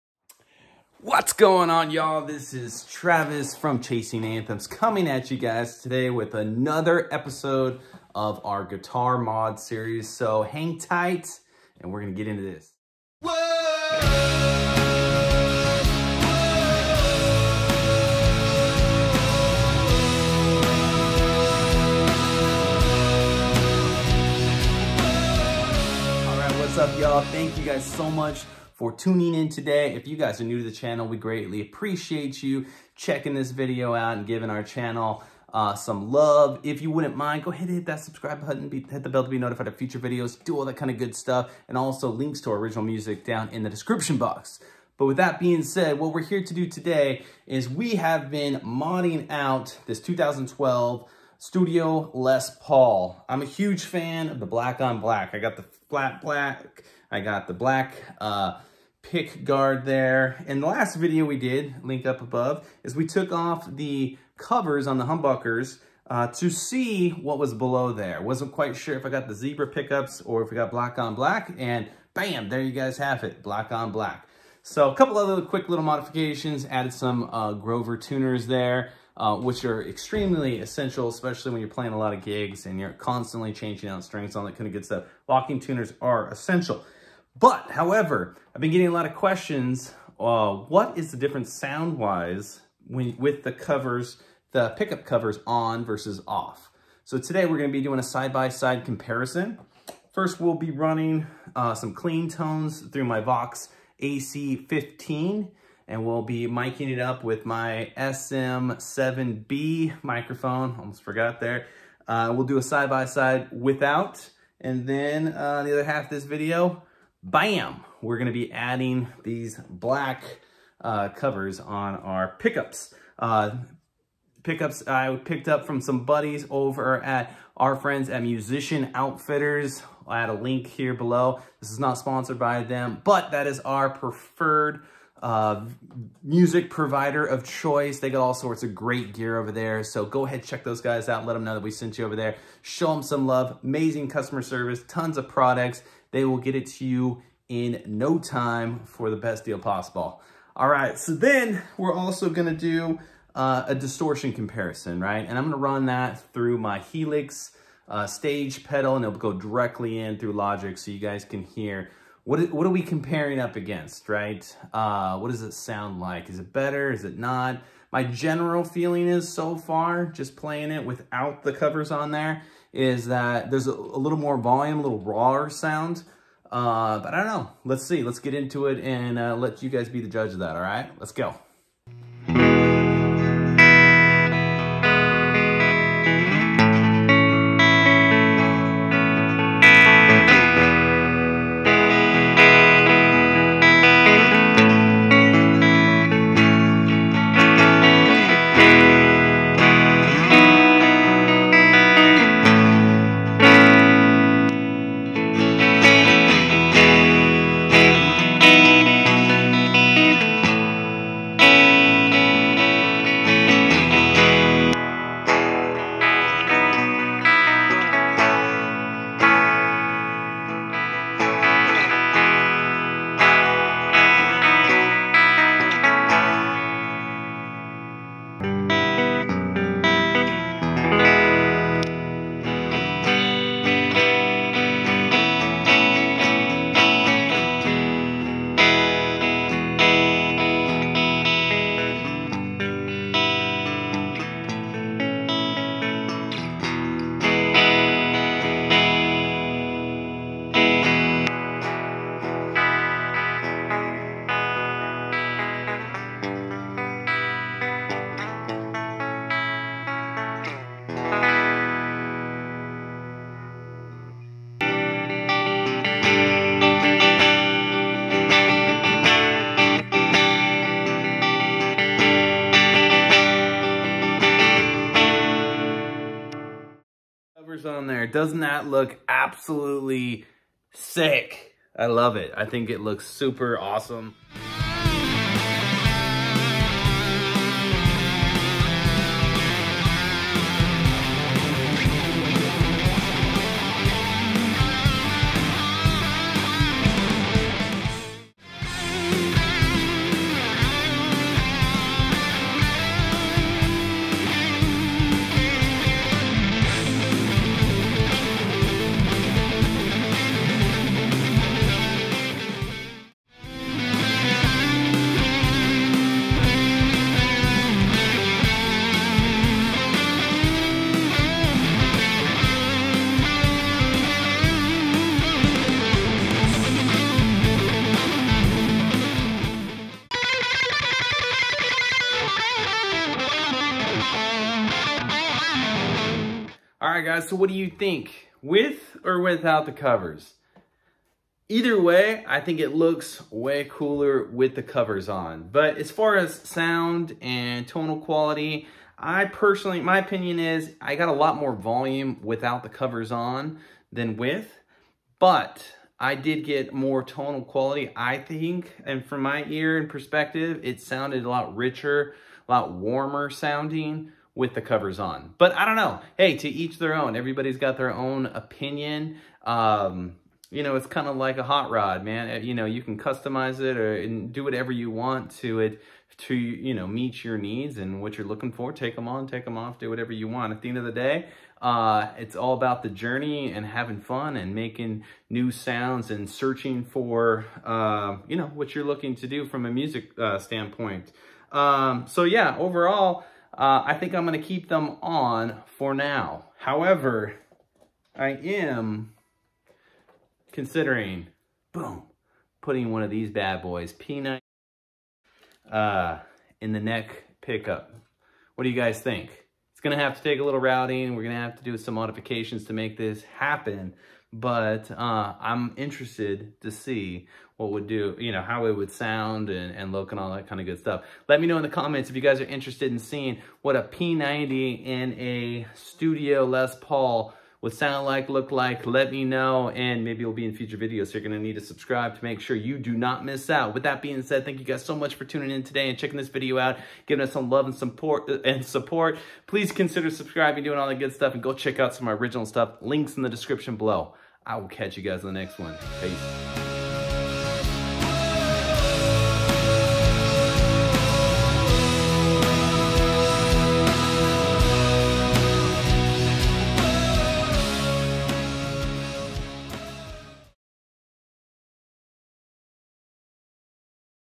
Does Removing the Pickup Covers Change the Sound of a Les Paul? | What Do You Think?
Shure SM7B Microphone
Beats Wireless Studio Headphones
Macbook Pro
Audio Interface